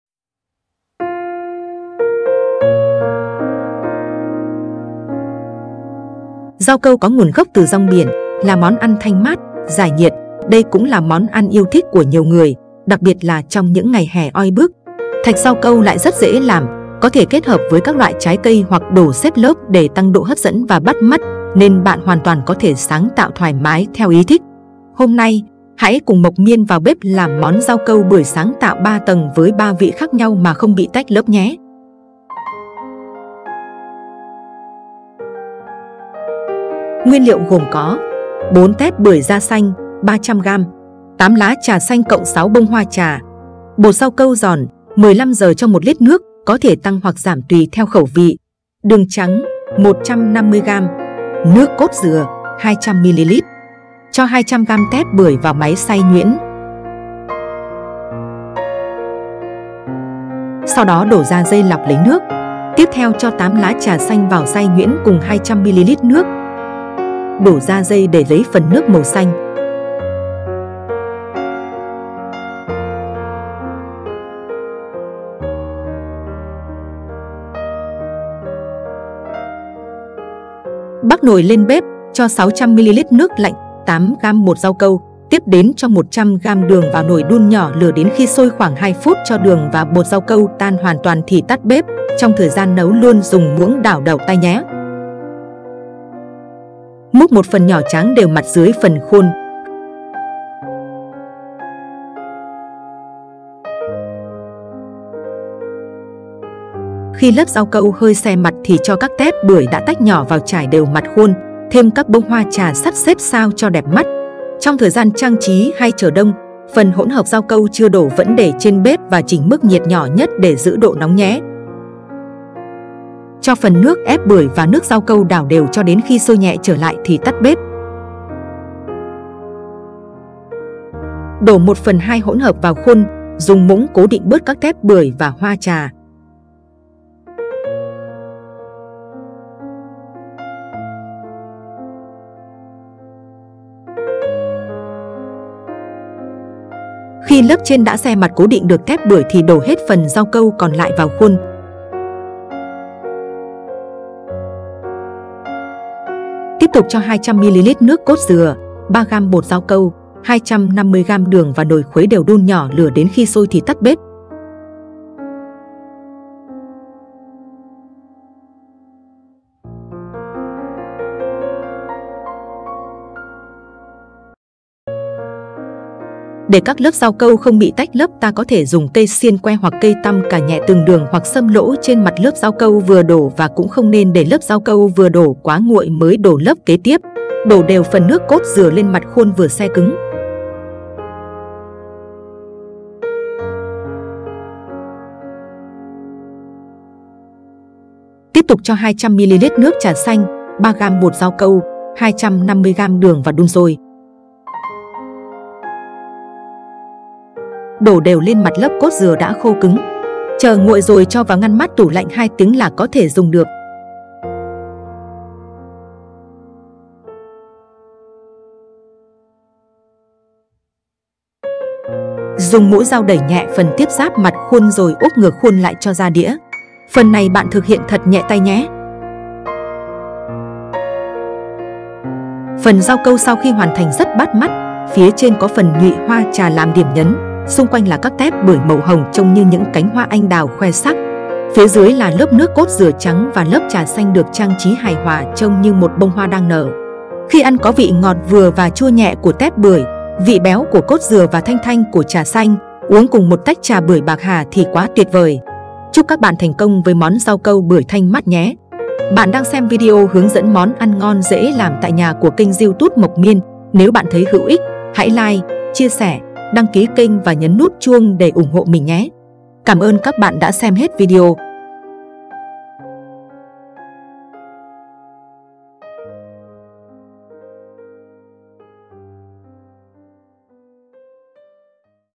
Món ngon mỗi ngày | MẸO LÀM RAU CÂU BƯỞI THANH MÁT 3 TẦNG MÀ KHÔNG BỊ TÁCH LỚP | Mộc Miên
Cảm ơn cả nhà đã xem hết video. Mong được làm quen và giao lưu với tất cả các bạn! ♥
Ghé mình chơi nhé!